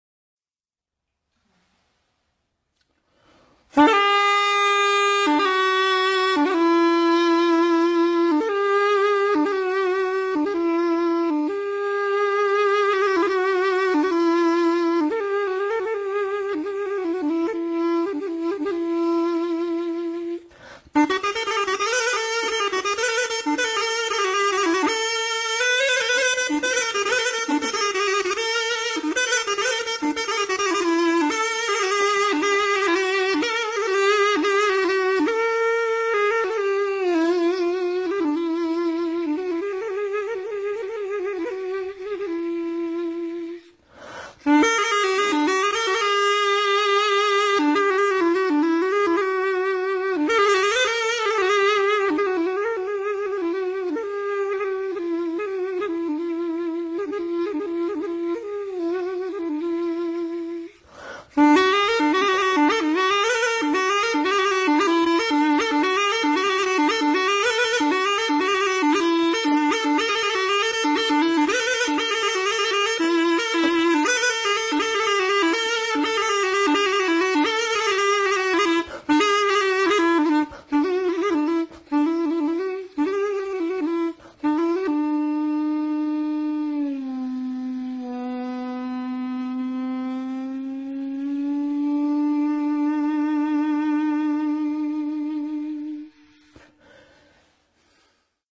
Флейта из Соломинки для Напитков. Drinking Straw Flute.
Пластиковая трубочка для напитков в руках маэстро становится  флейтой.